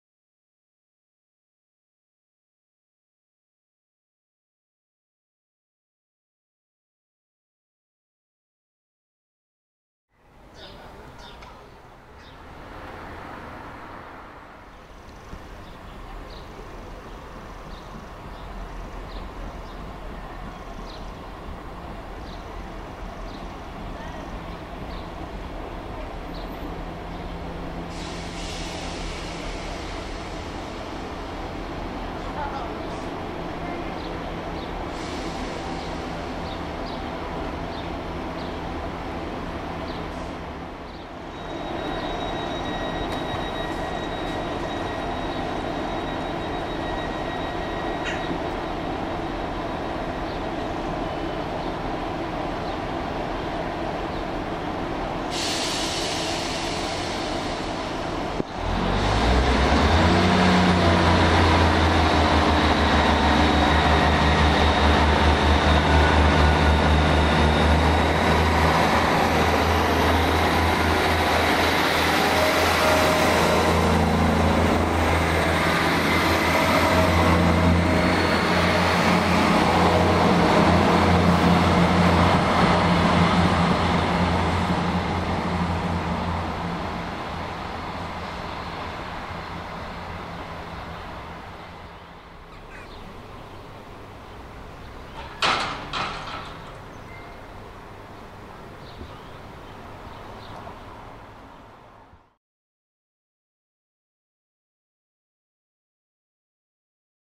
Celebrity GWR Unit 158957 at Pembrey & Burry Port 01/07/2018
GWR Class 158 unit has been causing some excitement across South Wales as it has been brought in by Arriva Trains Wales to deputise for it's own services.
With an oncoming Thunderstorm we see the unit arrive and depart from the Semaphore controlled Pembrey & Burry Port station whilst working the 11.25 Milford Haven to Manchester service.